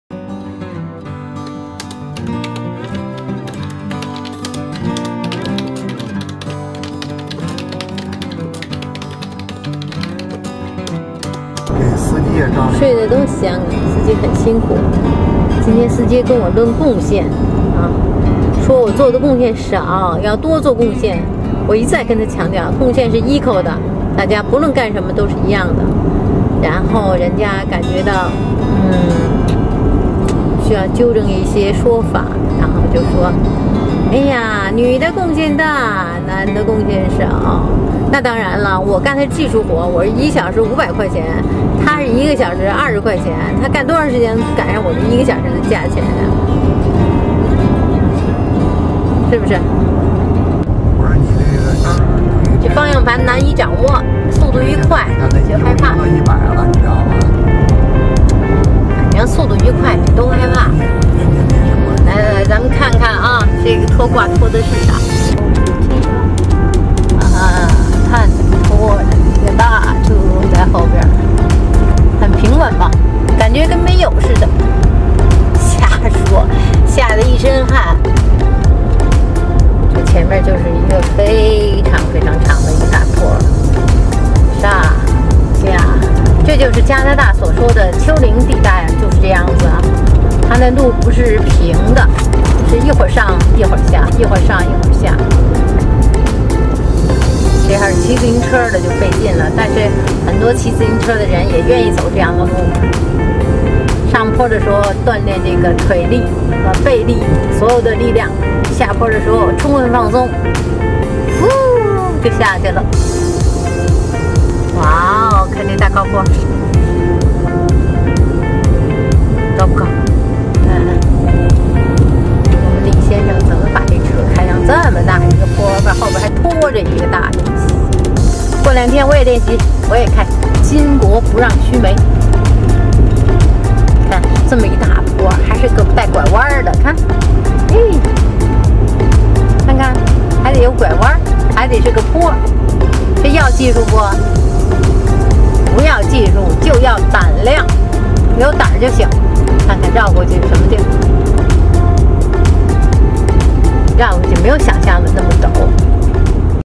评!评!理!流浪大篷车第一次,就论夫妻谁的贡献大?
流浪大蓬车第一次上路，第一次的兴奋，第一次的紧张兮兮，第一次论贡献